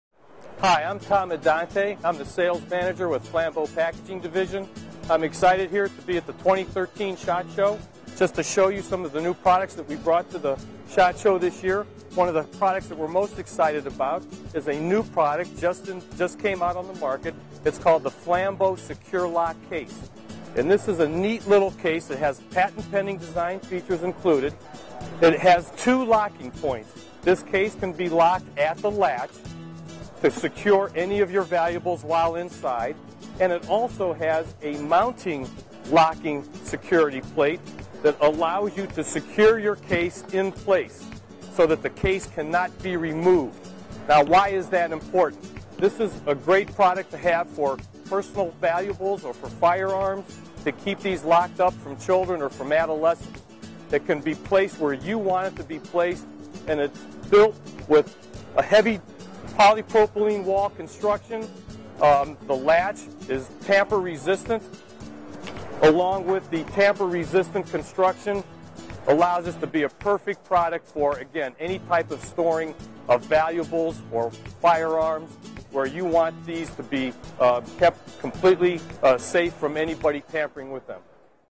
Flambeau Packaging Debuts New Secure-Lock Case at SHOT Show
Packaging Sales Manager, Tom Adante, introduces the new Secure-Lock Case at 2013 SHOT Show in Las Vegas, Nevada. The Secure-Lock Case is designed to help store and secure personal valuables and also includes a patent pending mounting feature as well.